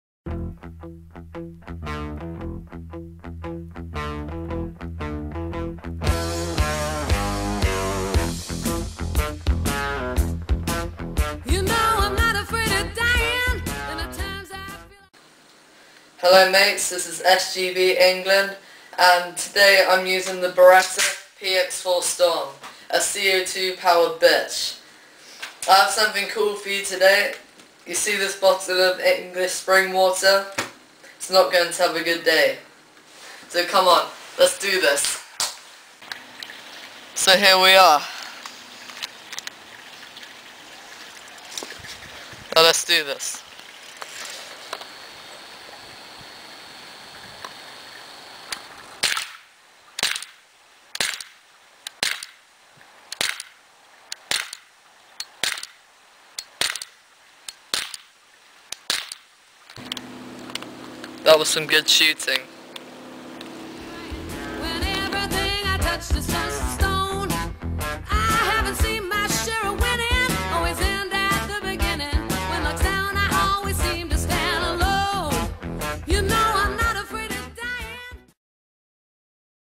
SGB England PT1
Beretta px4 storm destroying english water bottle
my impersonation of fps russia